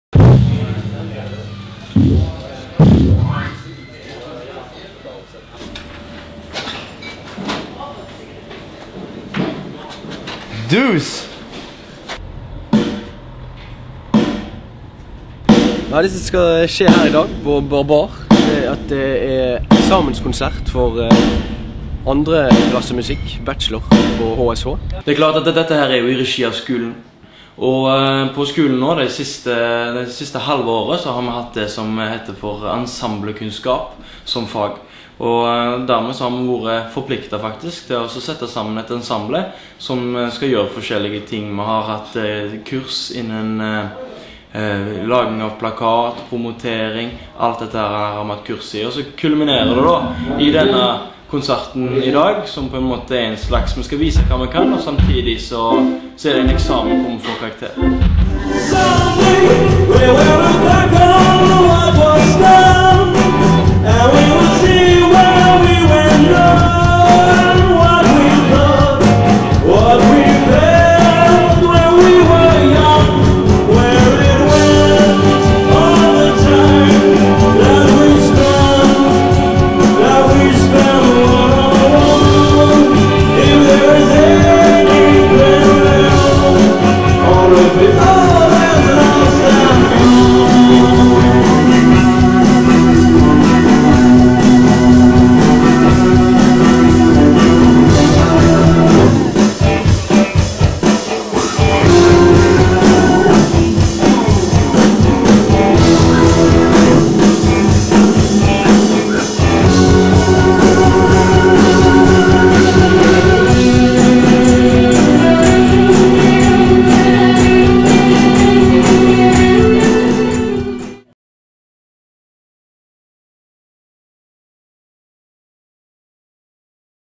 Musikkstudentar har eksamenskonsert
Musikkstudentar frå Høgskulen Stord/Haugesund arrangerer eksamenskonsert på Stord.